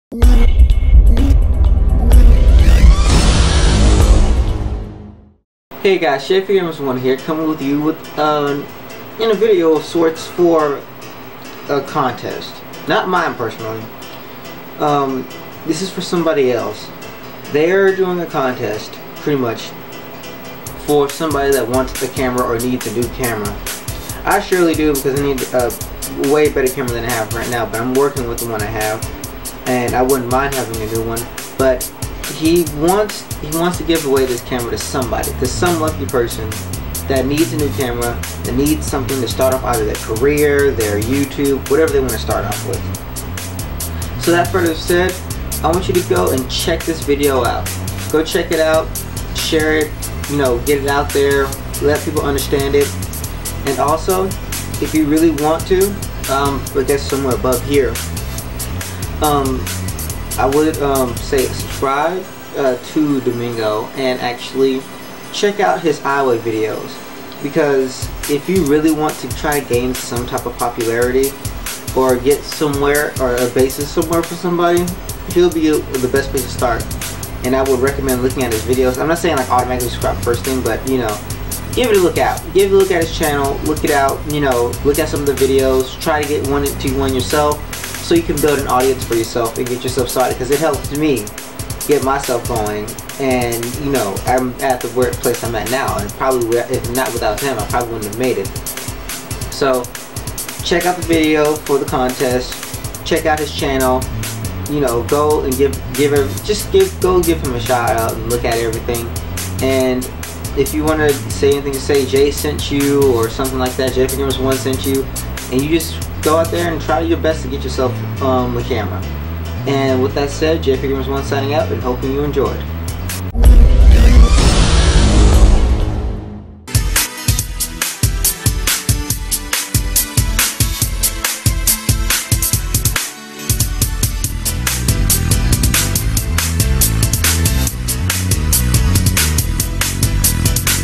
Want A Camera?
Here's a contest that can get you started or get you going for either your youtube career or just in general